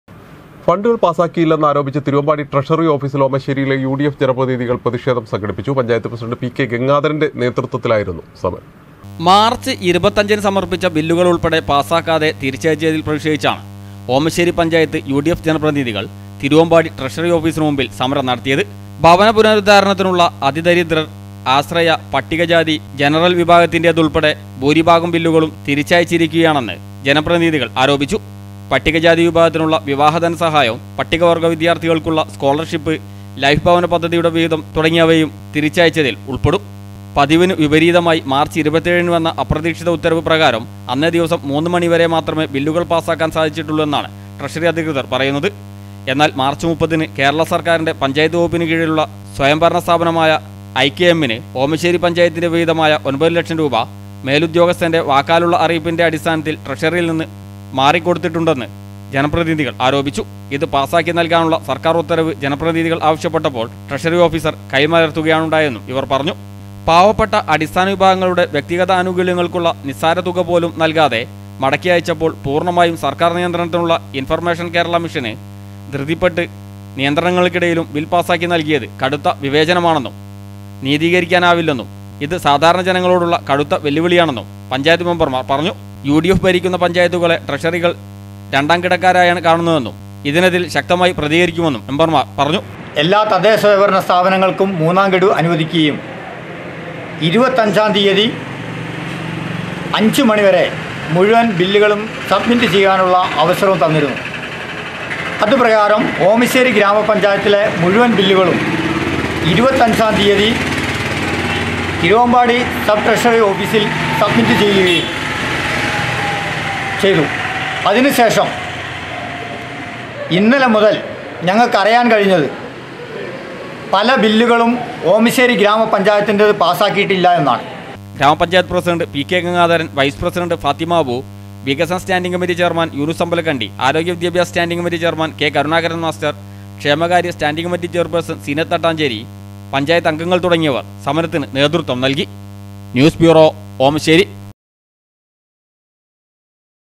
ഫണ്ടുകൾ പാസ്സാക്കിയില്ലന്നാരോപിച്ച്തിരുവമ്പാടി ട്രഷറി ഓഫീസിൽഓമശ്ശേരിയിലെയു.ഡി.എഫ്‌.ജനപ്രതിനിധികൾ
ഫണ്ടുകൾ പാസ്സാക്കിയില്ലന്നാരോപിച്ച്
തിരുവമ്പാടി ട്രഷറി ഓഫീസിൽ ഓമശ്ശേരിയിലെ യു.ഡി.എഫ്‌.ജനപ്രതിനിധികൾ പ്രതിഷേധം സംഘടിപ്പിച്ചു.